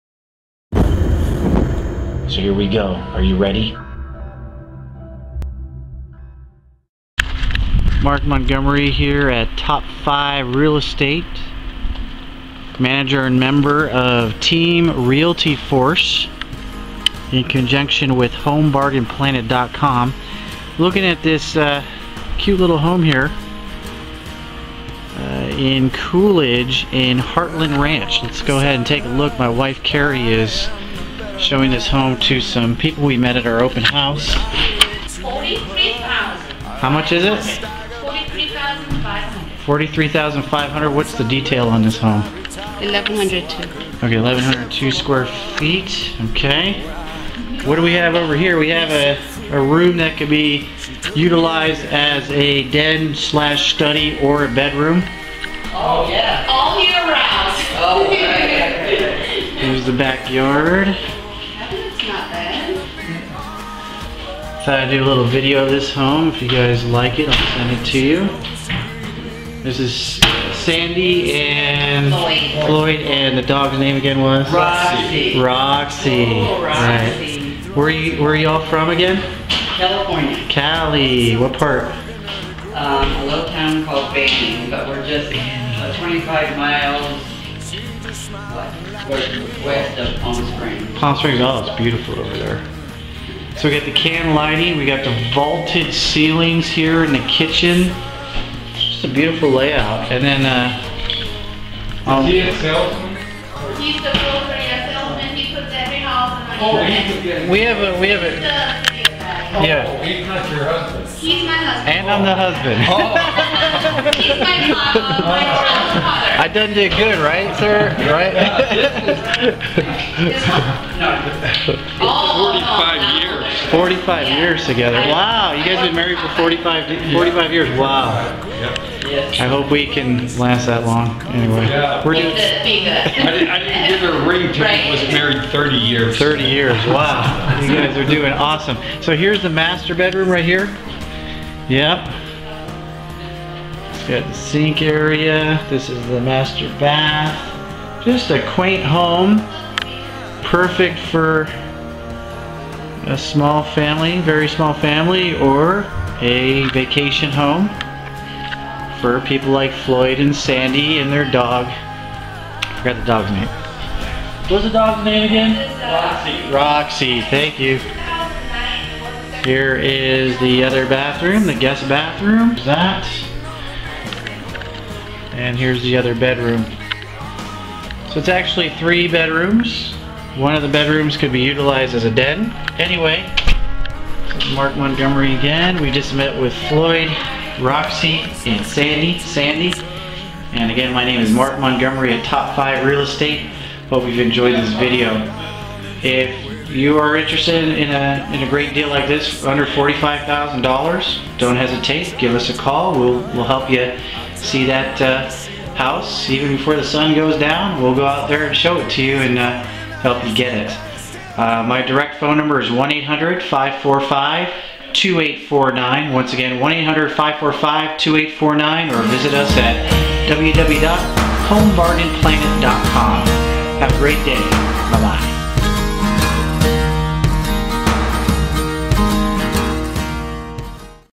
Coolidge, AZ - 3 Bedroom Single Level Home - $43,500! In Heartland Ranch
This home in Heartland Ranch of Coolidge, Arizona is quaint, but it's a home for under $45,000. With neighboring Casa Grande set to explode economically with the introduction of Phoenix Mart where close to 8000 jobs are predicted to open up, makes this home a wonderful investment opportunity. Take a look inside and if you are interested in getting hooked up like this, please call Team Realty Force of Top 5 Real Estate Group at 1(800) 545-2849. Thank you!